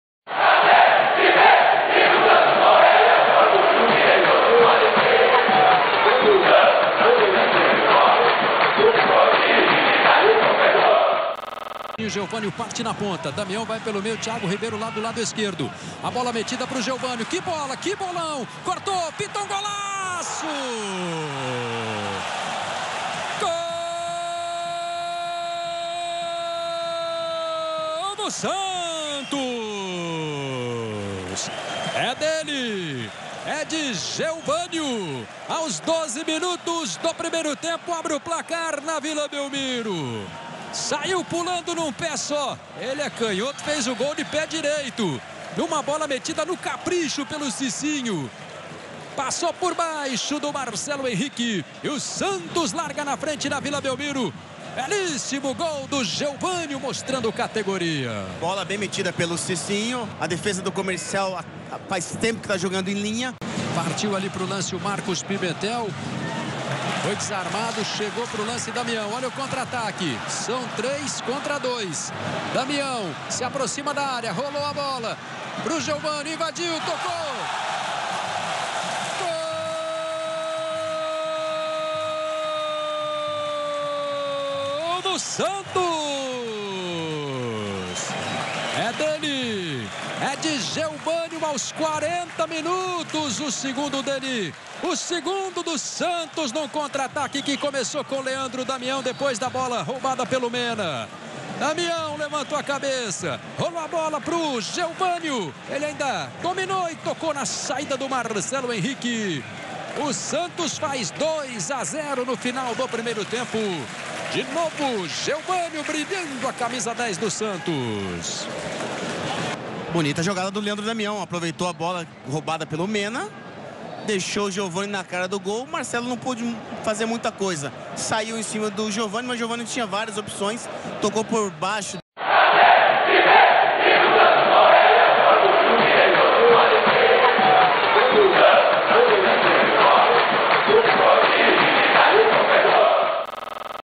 GOLS: Santos 2 x 0 Comercial - Campeonato Paulista 2014 11/02/2014 (Leão do Mar)
Gols da Vitória do Santos pra Cima do Comercial pelo Campeonato Paulista 2014
na Vila Belmiro

SEGUI NOSSO CANAL;